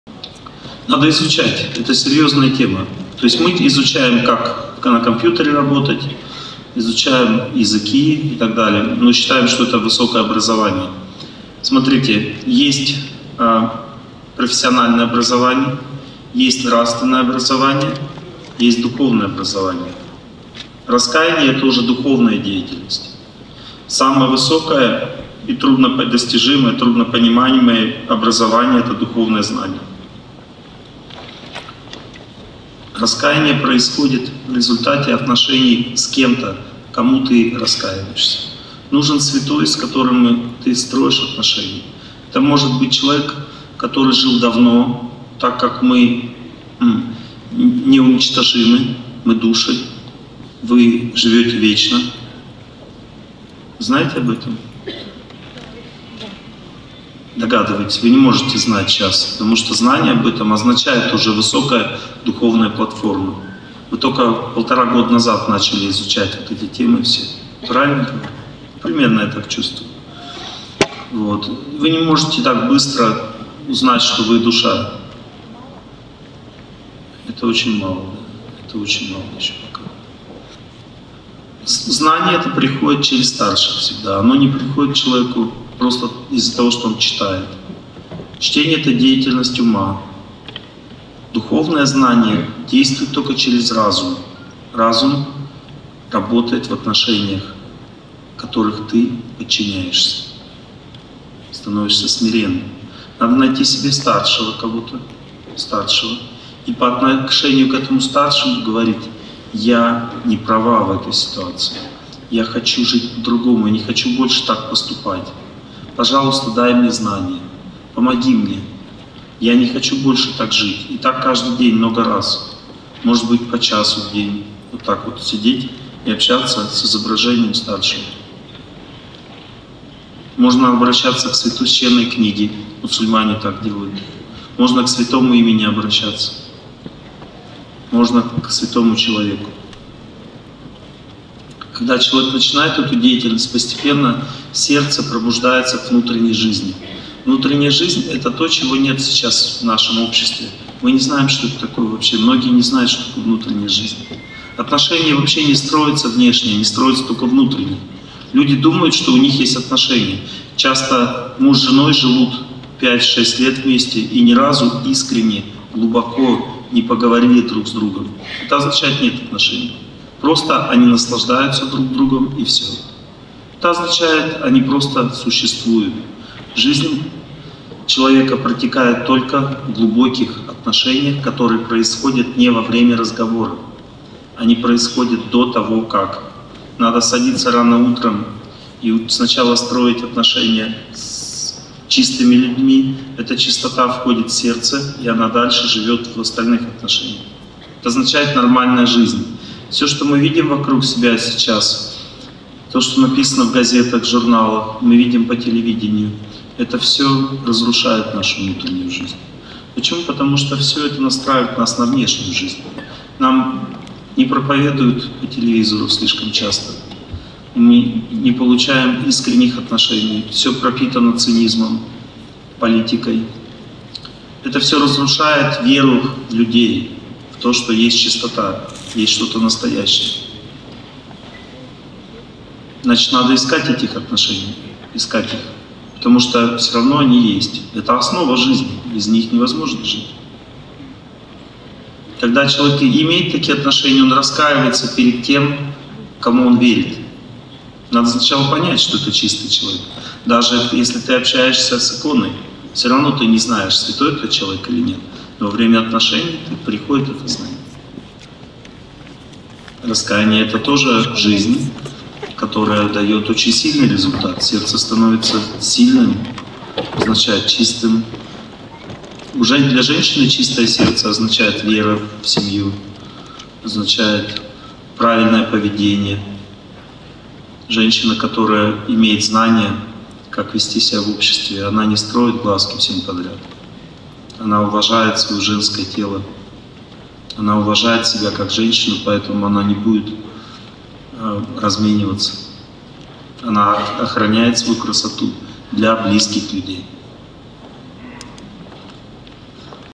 Торсунов О.Г. Как правильно раскаиваться
В этом видео говорится о как правильно раскаиваться
Из лекции "Таинство зачатия и рождения" Москва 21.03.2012